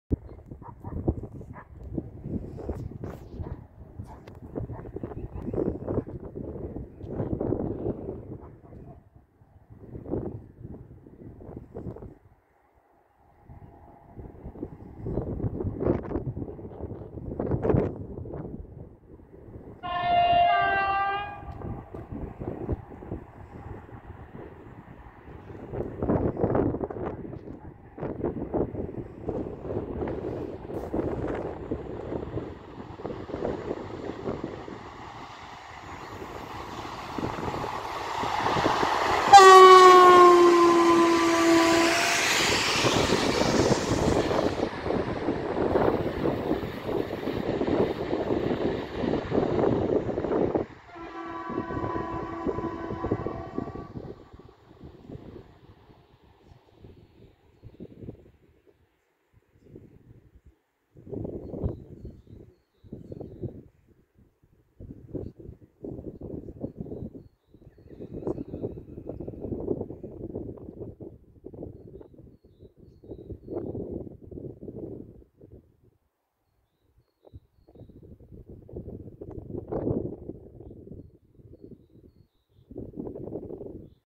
IC "San" w przelocie z efektem Dopplera
Pociąg IC "San" relacji Warszawa - Przemyśl szybko przejeżdża przez stację Wąchock.
Podziękowania dla maszynisty za Rp 1 z efektem Dopplera!